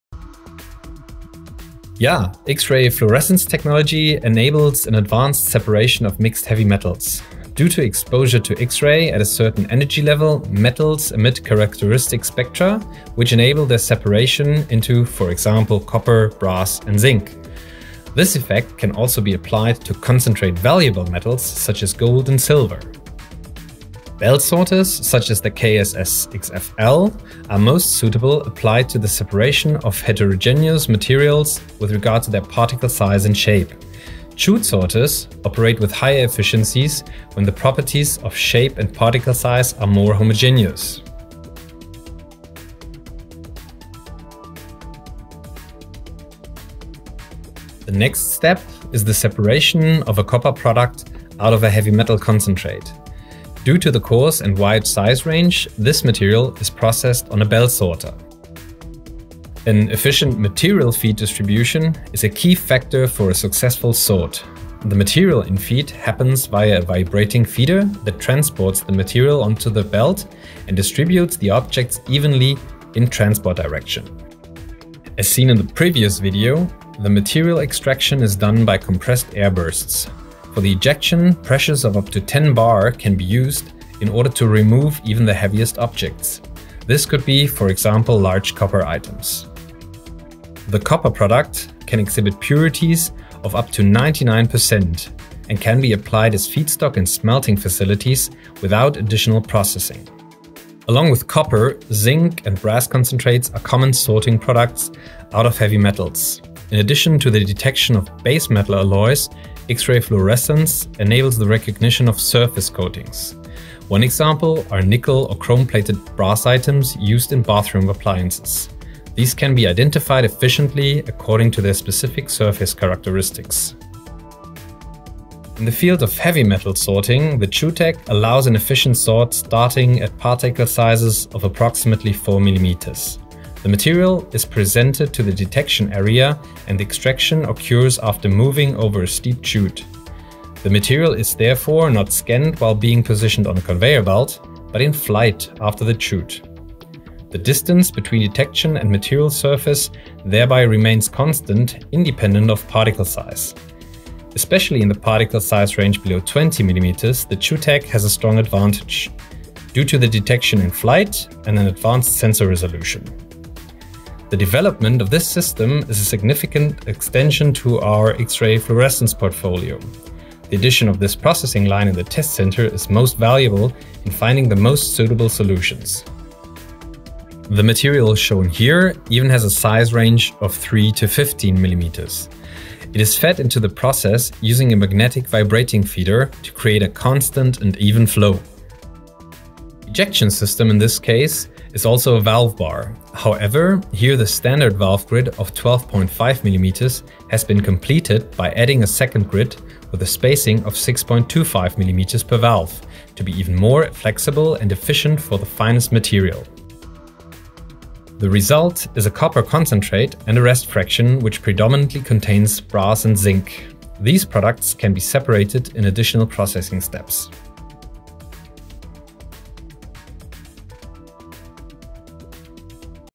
X-ray fluorescence (XRF) technology
The video clip of the digital live opening of the new STEINERT Test and Development Center shows sorting solutions based on X-ray fluorescence (XRF) technology.